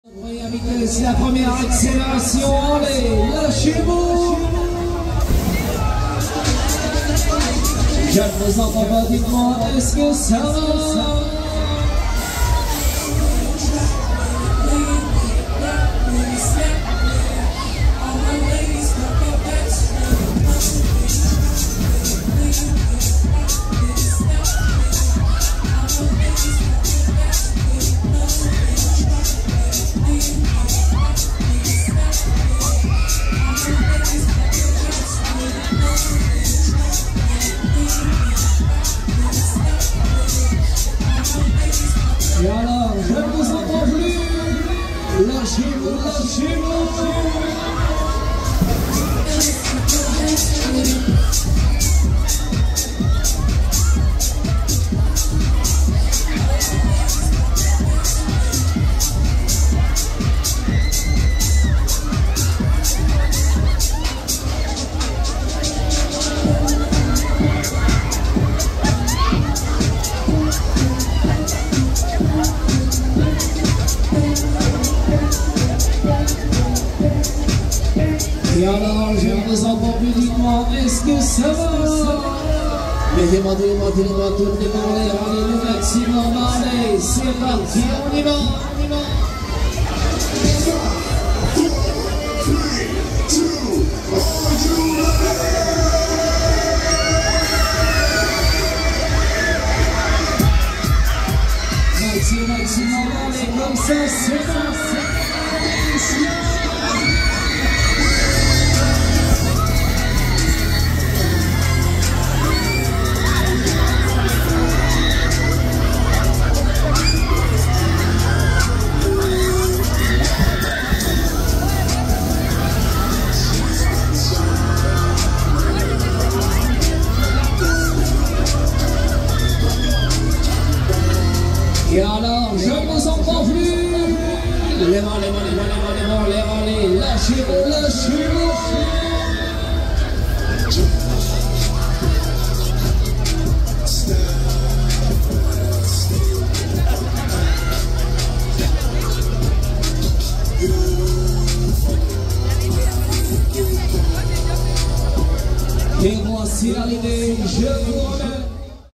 SHAKE OFF - Foires Franches Brive-La-Gaillarde 2024
Le Shake Off (Direction Pierdon) sur la fête foraine des Foires Franches de Brive-La-Gaillarde (19) 2024.